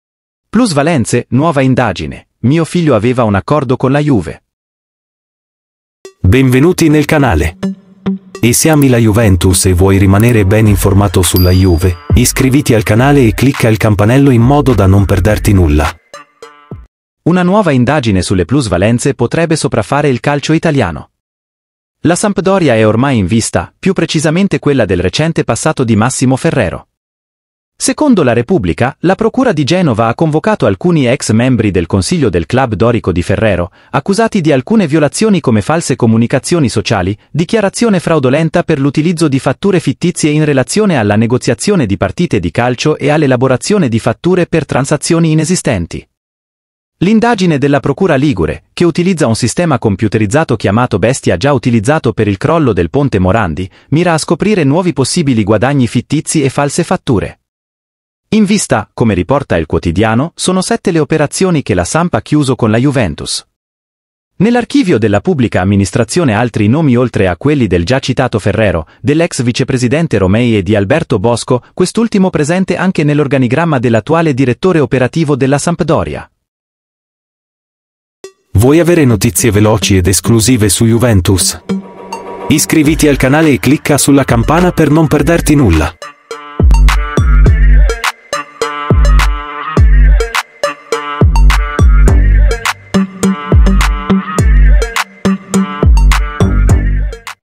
mio figlio aveva un accordo con la Juve..// [NOTIZIE DA JUVENTUS]
Una nuova indagine sulle plusvalenze potrebbe sopraffare il calcio italiano. La Sampdoria è ormai in vista, più precisamente quella del recente passato di Massimo Ferrero.
Secondo La Repubblica, la Procura di Genova ha convocato alcuni ex membri del consiglio del club dorico di Ferrero, accusati di alcune violazioni come false comunicazioni sociali, dichiarazione fraudolenta per l'utilizzo di fatture fittizie in relazione alla negoziazione di partite di calcio e all'elaborazione di fatture per transazioni inesistenti.

L'indagine della Procura Ligure, che utilizza un sistema computerizzato chiamato 'Bestia' già utilizzato per il crollo del ponte Morandi, mira a scoprire nuovi possibili guadagni fittizi e false fatture. In vista, come riporta il quotidiano, sono sette le operazioni che la Samp ha chiuso con la Juventus.
Nell'archivio della Pubblica Amministrazione altri nomi oltre a quelli del già citato Ferrero, dell'ex vicepresidente Romei e di Alberto Bosco, quest'ultimo presente anche nell'organigramma dell'attuale direttore operativo della Sampdoria..